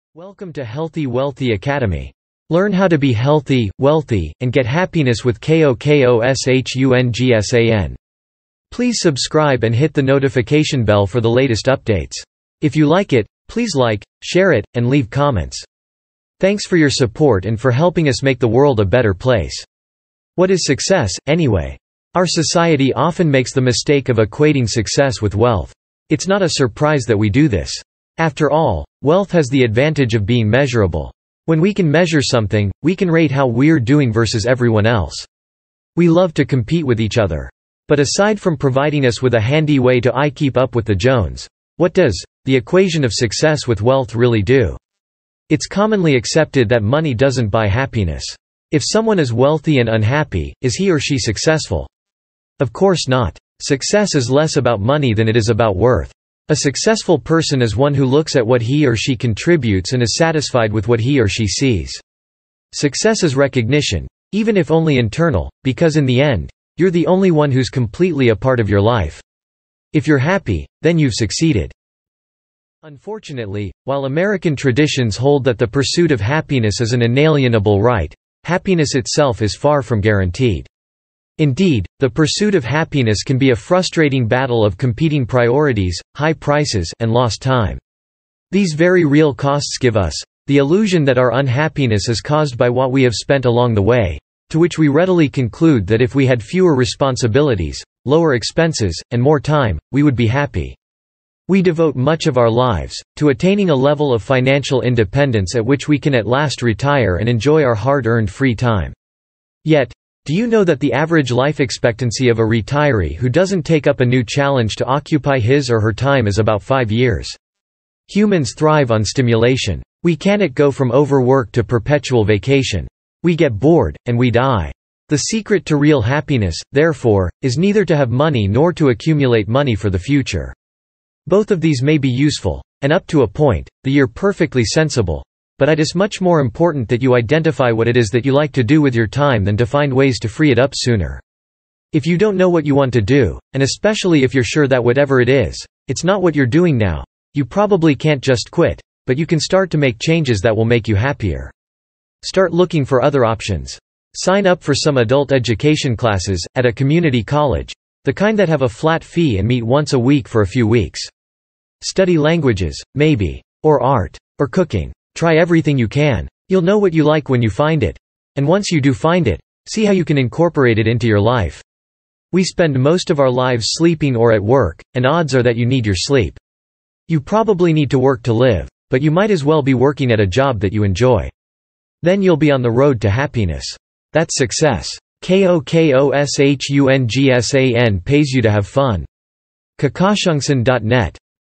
What is success, anyway?
What is success, anyway? Virtually everyone strives for success, but few people have an idea of what they are struggling to attain. Oftentimes, success is confused with wealth, while other people seek a life of complete leisure, only to be disappointed with either outcome.

TIMESTAMPS
00:00 Intro​
00:20 What is success, anyway?
03:00 Final

💗KOKOSHUNGSAN® Pays You To Have Fun! The Fun Never Stops!